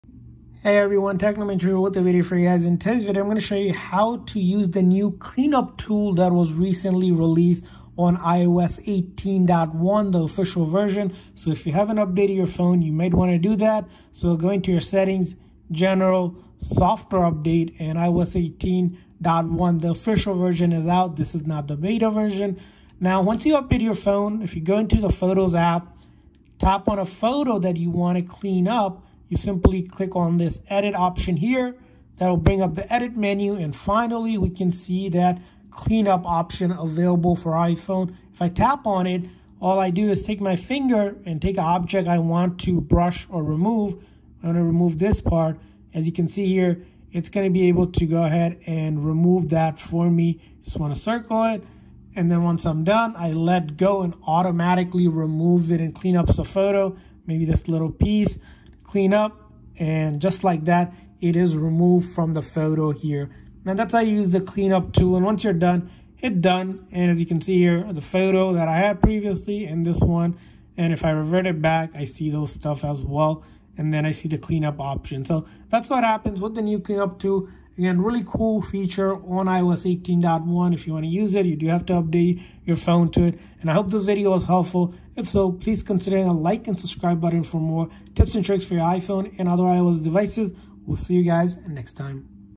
How To Use Clean Up Tool on iOS 18 (New Feature!)
iOS 18.1 has a new feature called Clean Up Tool to remove background objects. In this video I show you how to use the Clean Up Tool on Photos app on iOS 18 on iPhone.